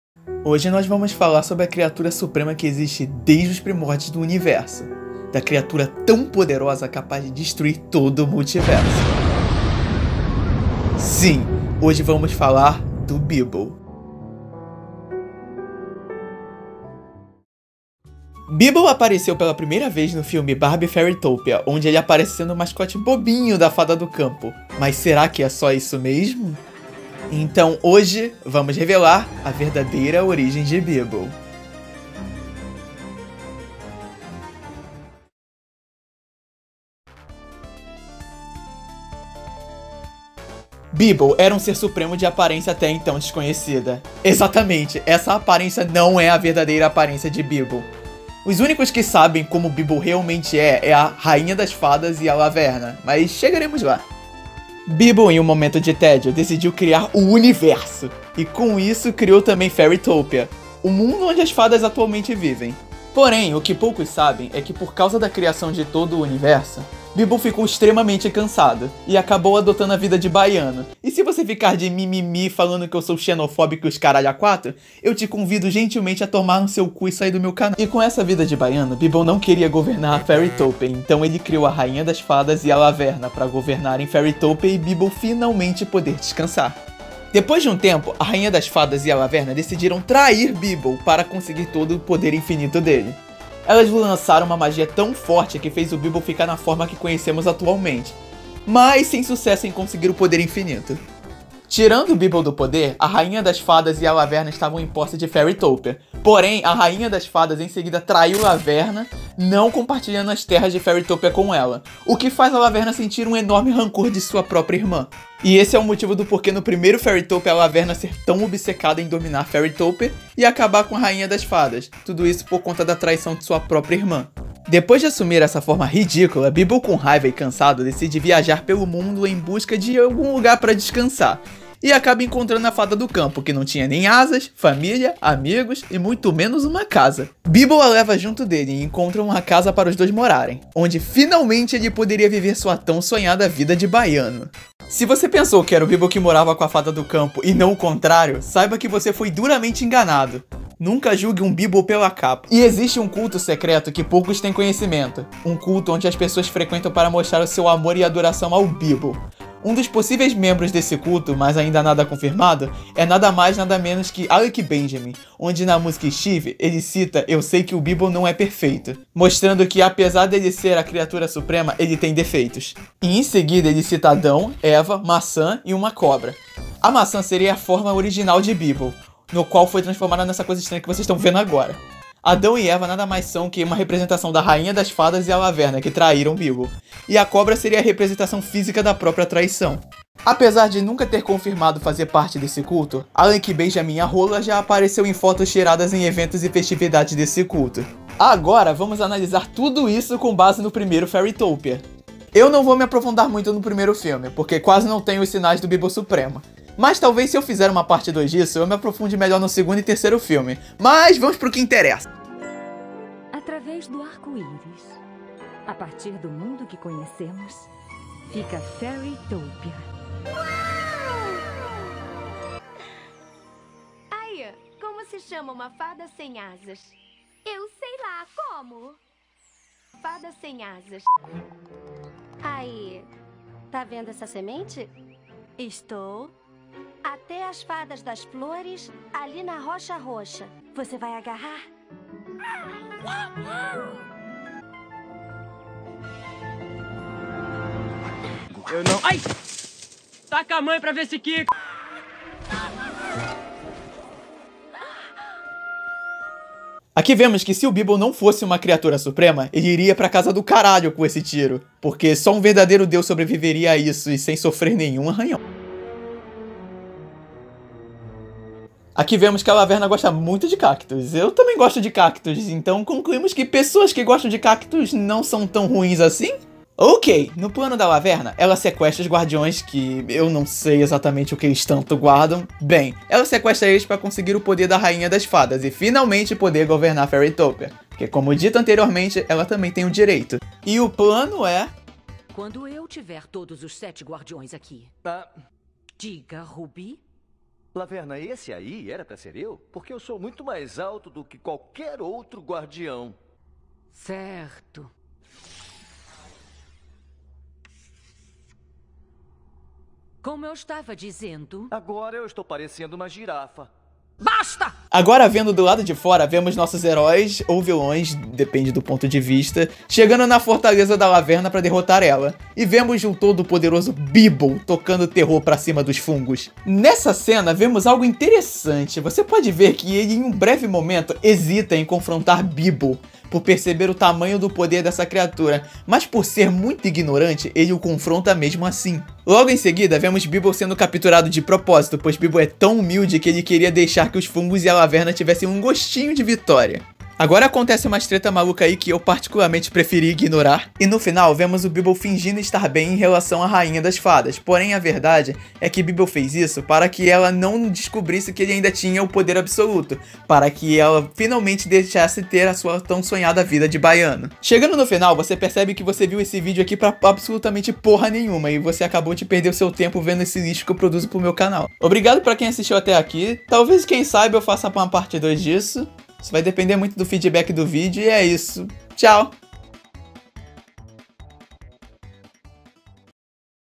A criatura que não foi aceita nem pelo céu e nem pelo inferno
Hoje eu vim dizer um pouco da palavra do grande poderoso Bibble, contar um pouco da sua história e mostrar como a mente de 2 pessoas podem fazer um vídeo tosco igual esse.